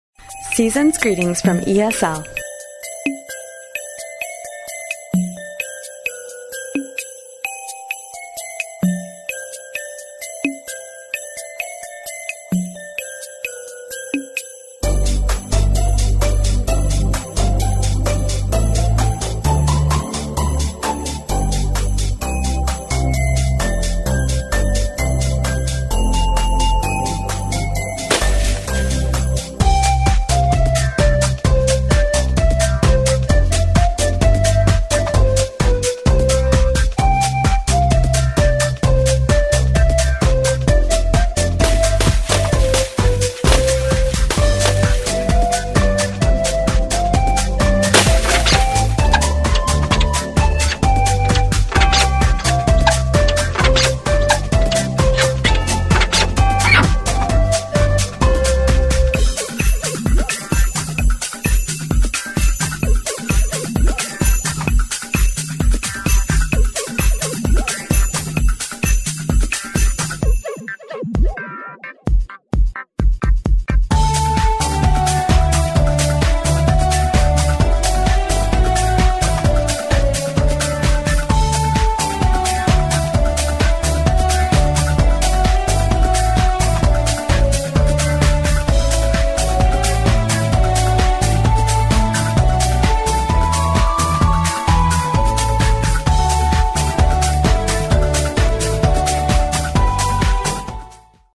ESL Family 2012 Christmas eCard
Happy Holidays from the ESL Family of Companies! Watch the very animated ESL Family reenact a short, comical spoof of the classic holiday movie, "A Christmas Story". Make sure your Leg Lamp is positioned properly so you don't miss a second.